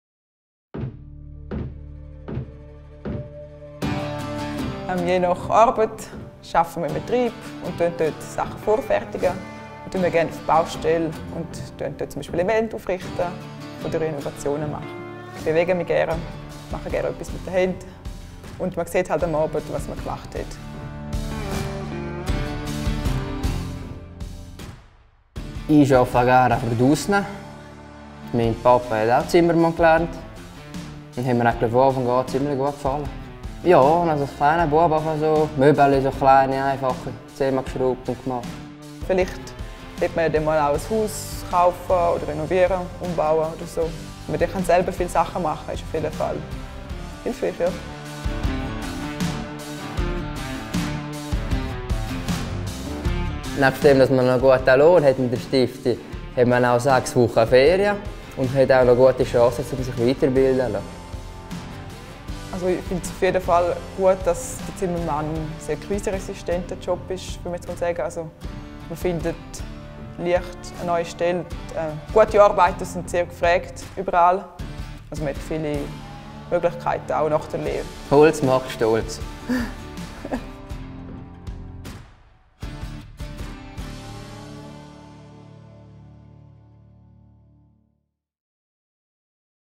Eine Lehre als Zimmermann/Zimmerin - Holz macht stolz!
Ein Einblick in den Alltag einer Lehrstelle als Zimmermann/Zimmerin EFZ.